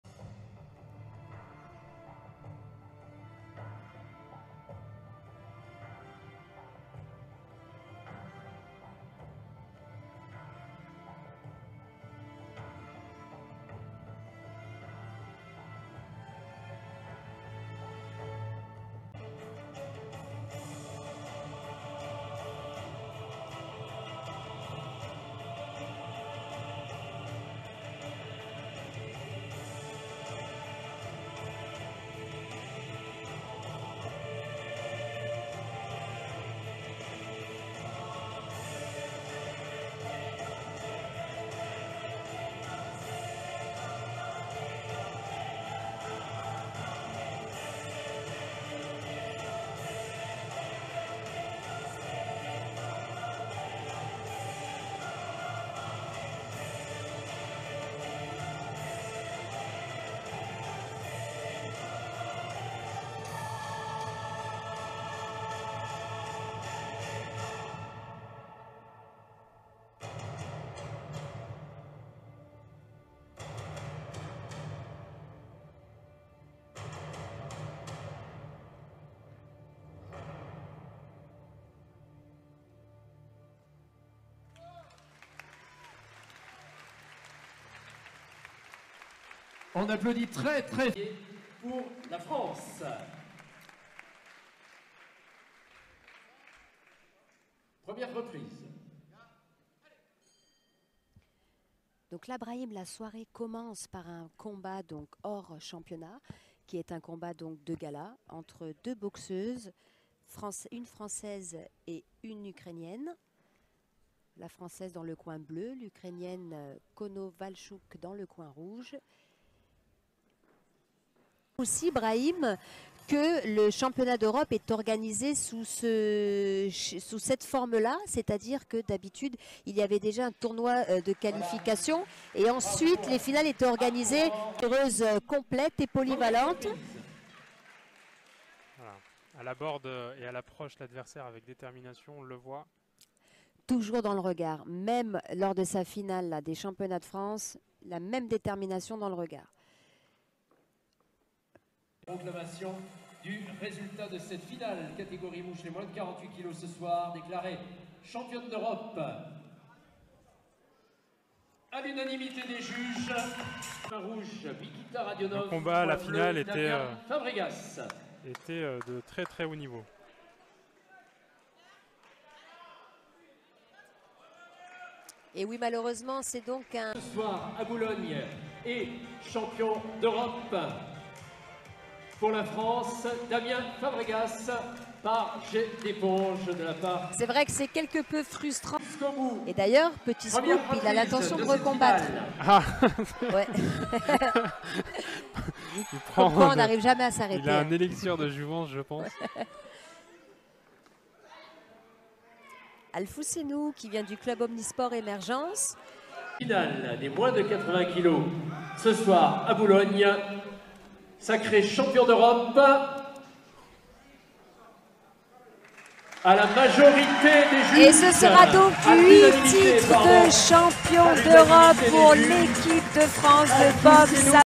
Чемпионат Европы по савату во Франции 10.09.2022.
Странные впечатления по Чемпионату Европы по савату 2022 года.